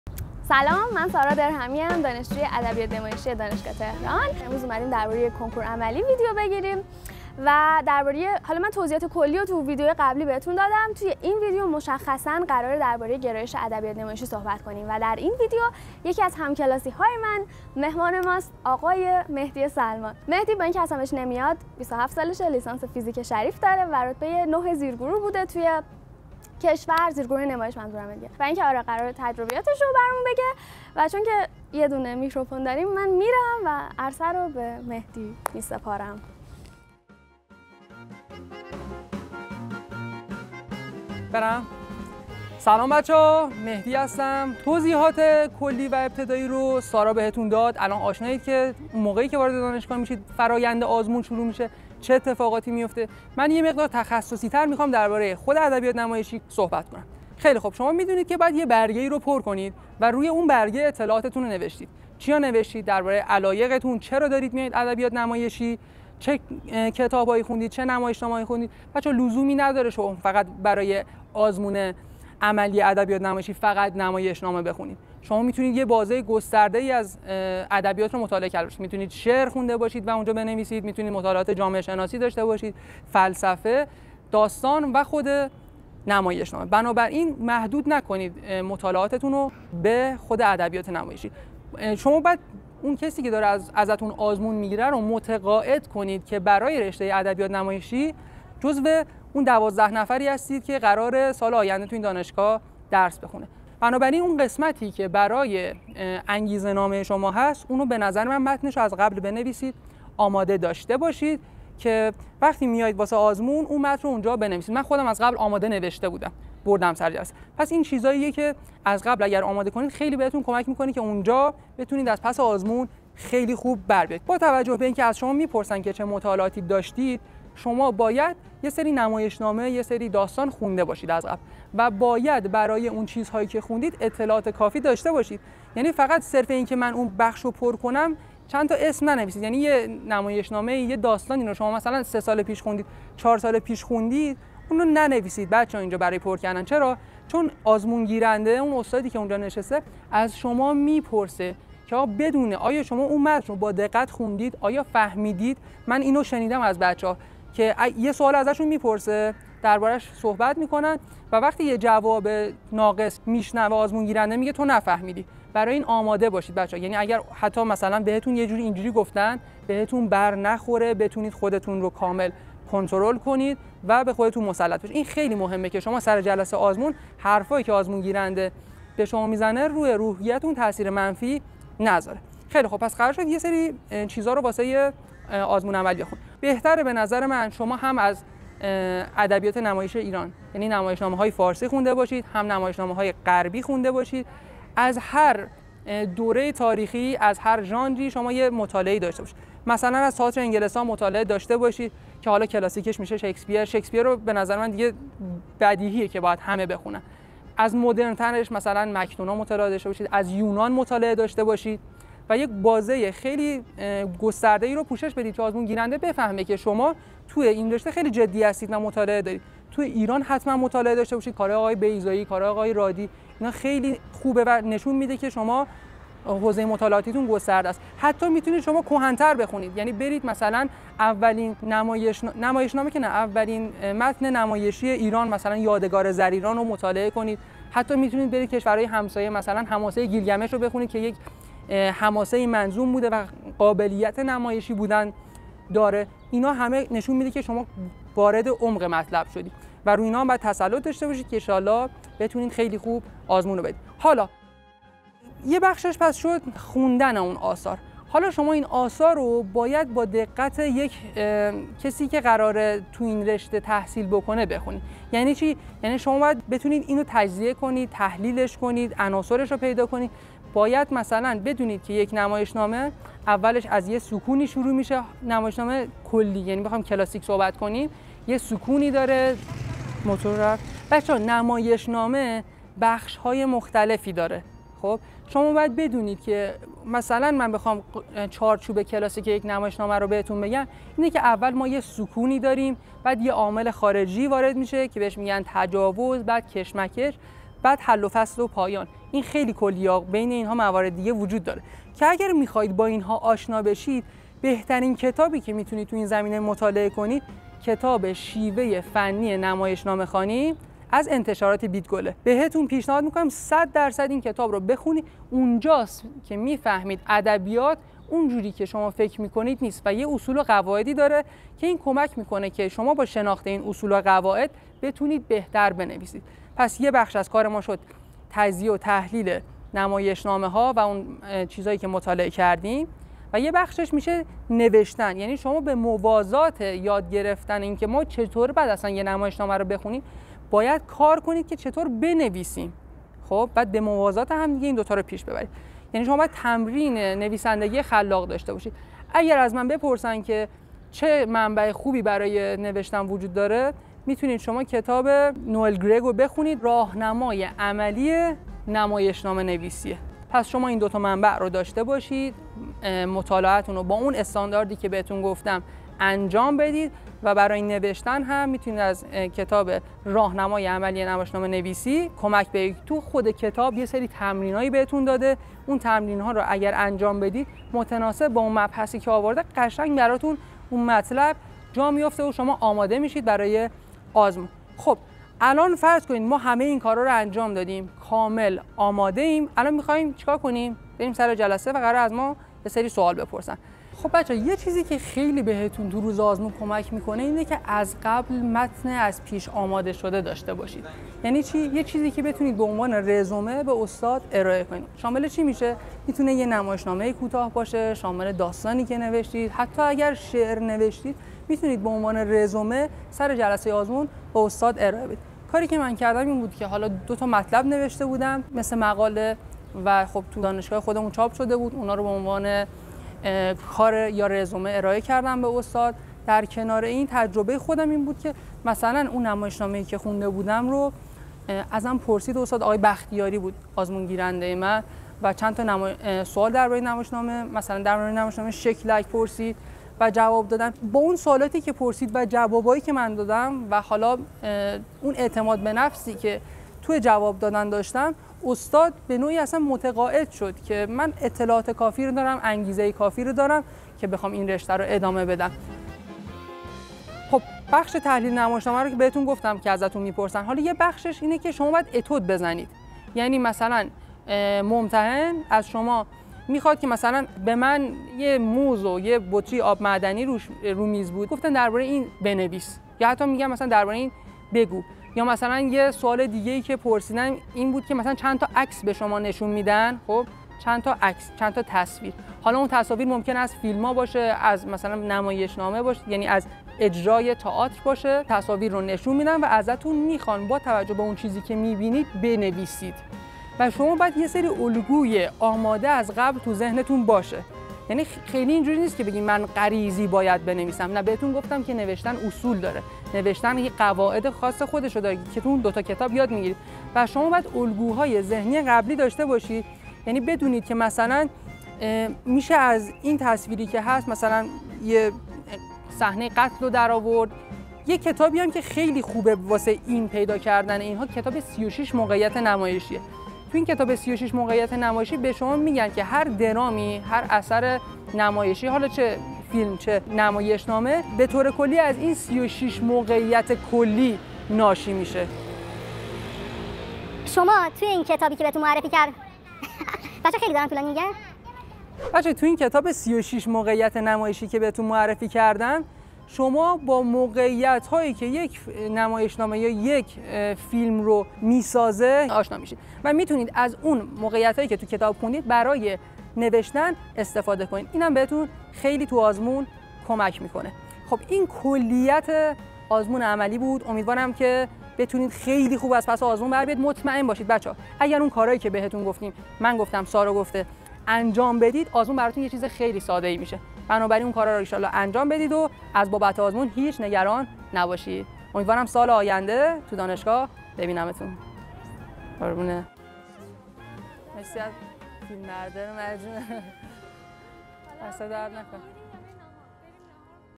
کنکور عملی ادبیات نمایشی، صفرتاصد: قبولی در پرطرفدارترین گرایش تئاتر
تو این ویدیو مهدی سلمان، مشاور کنکور هنر، براتون درباره‌ی آزمون عملی ادبیات نمایشی توضیح می‌ده
قبل از این ویدیو توضیحات کلی رو ببینید که اینجا گفتم

00:00 | قصه چیه؟
00:58 | چی خونده باشیم؟
02:06 | اهمیت ضدگلوله بودن
03:05 | از چه دوره‌ای بخونیم؟
04:15 | آیا باید به شکل خاصی بخونیم؟
05:01 | کتاب عالی برای یادگیری تحلیل نمایشنامه
05:45 | تمرین نویسندگی با آقای گرگ
06:25 | روز آزمون چه با خود ببریم؟
07:36 | چه اتودی می‌خوان؟
08:42 | کتاب کمک‌کننده برای موقعیت‌یابی
09:33| پایه دوربین نرم و رایگان